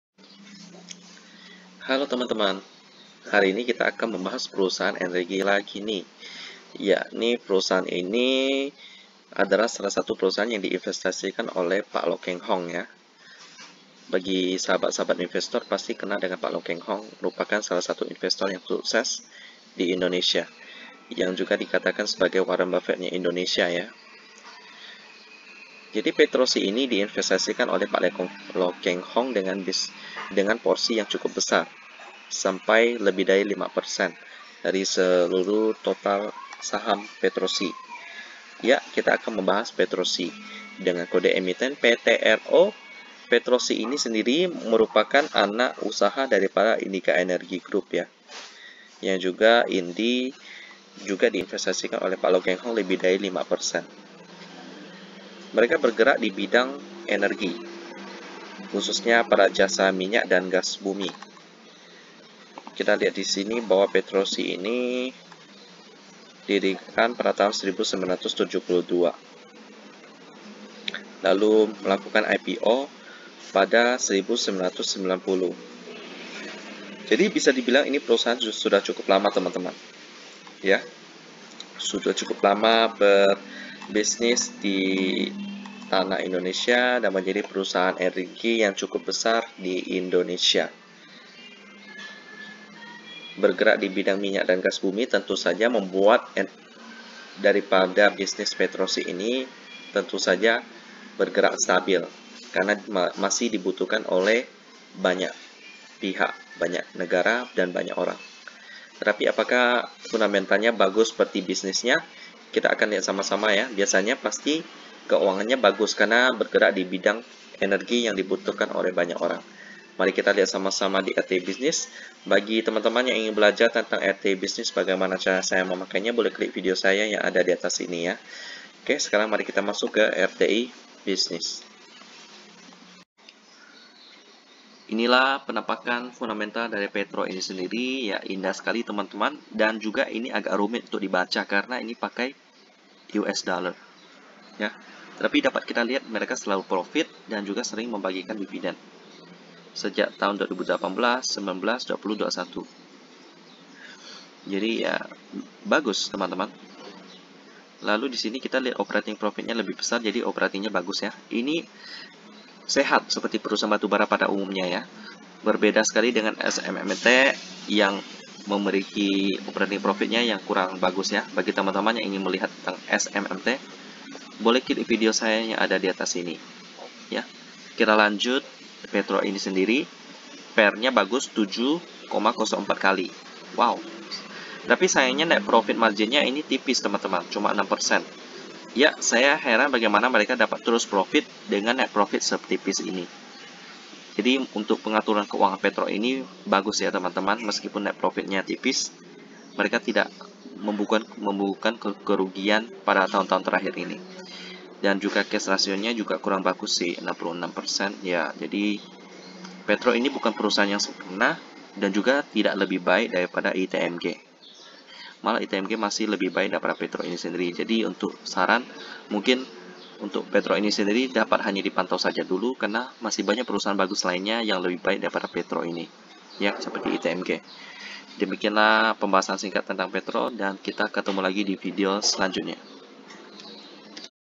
Petrosea Tbk saham (PTRO) cari cuan dari energi...
merupakan saham yang bergerak di bidang minyak bumi dan gas...